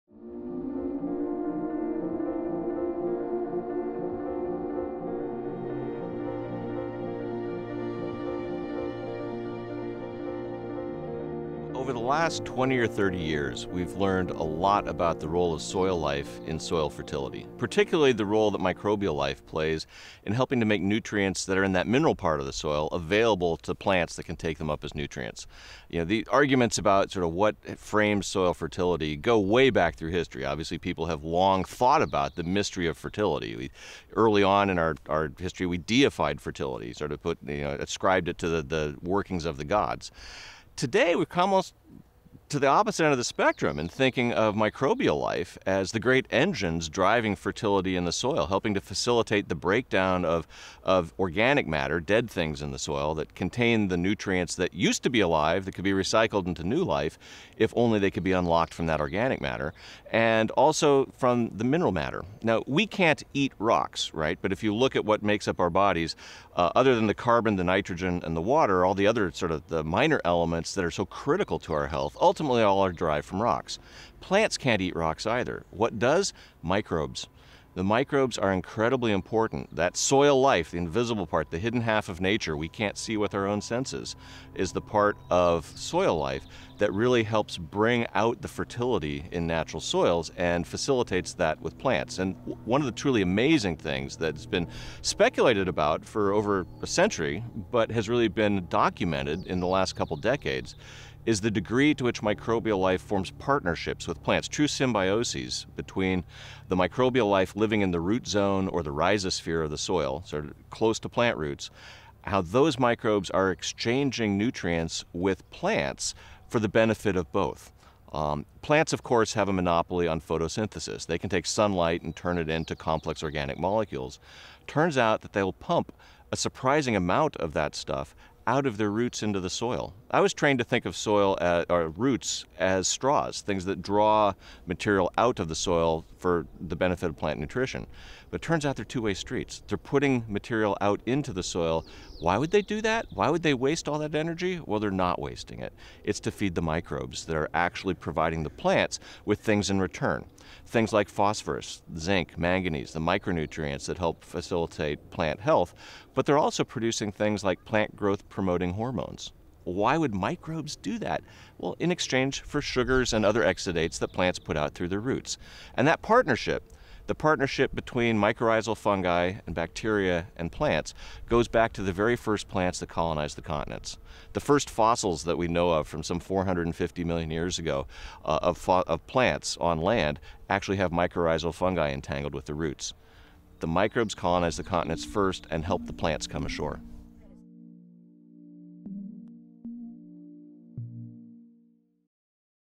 David R. Montgomery on Symbioses in the Soil
David Montgomery, co-author of The Hidden Half of Nature and Professor of Geomorphology at the University of Washington, describes the amazing symbioses between plants and microbes in the soil.

Transcript: Over the last 20 or 30 years we’ve learned a lot about the role of soil life in soil fertility. Particularly the role that microbial life plays in helping to make nutrients that are in that mineral part of the soil available to plants that can take them up as nutrients. The arguments about sort of what frames soil fertility go way back through history. Obviously people have long thought about the mystery of fertility. Early on in our history we deified fertility, ascribed it to the workings of the gods. Today we’ve come almost to the opposite end of the spectrum in thinking of microbial life as the great engines driving fertility in the soil, helping to facilitate the breakdown of organic matter—dead things in the soil—that contain the nutrients that used to be alive that can be recycled into new life if only they could be unlocked from that organic matter. And, also from the mineral matter. Now, we can’t eat rocks, right? But if you look at what makes up our bodies, other than the carbon, the nitrogen, and the water, all the other sort of minor elements that are so critical to our health ultimately all are derived from rocks. Plants can’t eat rocks either. What does? Microbes. The microbes are incredibly important. That soil life, the invisible part, the hidden half of nature we can’t see with our own senses is the part of soil life that really helps bring out the fertility in natural soils and facilitates that with plants. One of the truly amazing things that’s been speculated about for over a century but has really been documented in the last couple decades is the degree to which microbial life forms partnerships with plants. True symbioses between the microbial life living in the root zone, or the rhizosphere of the soil—sort of, close to plant roots—how those microbes are exchanging nutrients with plants for the benefit of both. Plants of course have a monopoly on photosynthesis. They can take sunlight and turn it into complex organic molecules. Turns out that they’ll pump a surprising amount of that stuff out of their roots into the soil. I was trained to think of soil, or roots, as straws—things that draw material out of the soil for the benefit of plant nutrition. But it turns out they’re two-way streets. They’re putting out material into the soil. Why would they do that? Why would they waste all that energy? Well they’re not wasting it. It’s to feed the microbes that are actually providing the plants with things in return. Things like phosphorous, zinc, manganese, the micronutrients that help facilitate plant health. But they’re also producing things like plant-growth promoting hormones. Why would microbes do that? Well, in exchange for sugars and other exudates that plants put out through their roots. And that partnership, the partnership between mycorrhizal fungi and bacteria and plants goes back to the very first plants that colonized the continents. The first fossils that we know of from some 450 million years ago, of plants on land, actually have mycorrhizal fungi entangled with the roots. The microbes colonized the continents first and helped the plants come ashore.